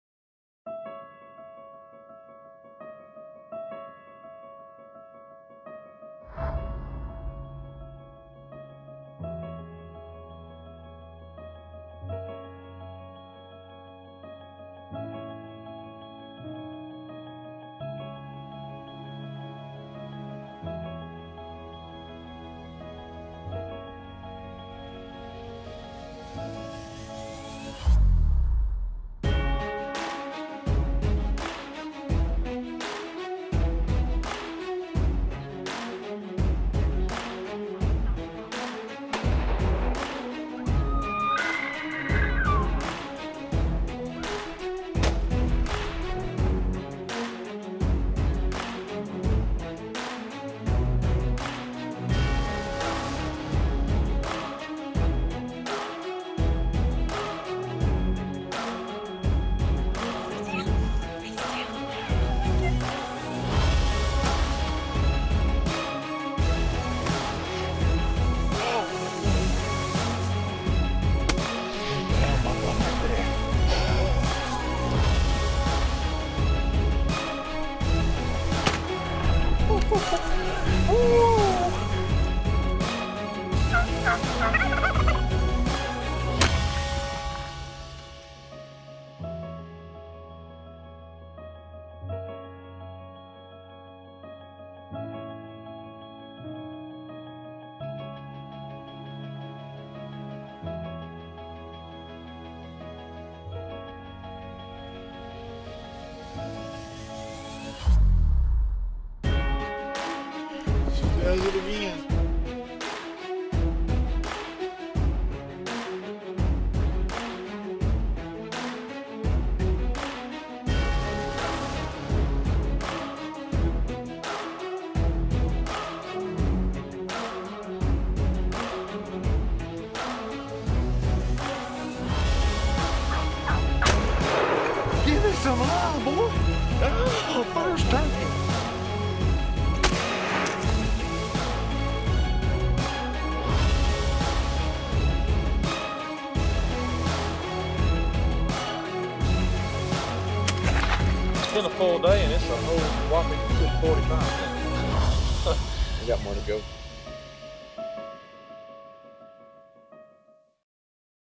2017 Chasin Creation Promo
After being with Bill Winke's Midwest Whitetail team the past several years, Chasin Creation has been blessed to experience some amazing #‎OutdoorAdventures. With several #‎AwesomeHunts and #‎FishingTrips behind us, it's time to share them with the hunting world. This video will give you a glimpse of what is to come.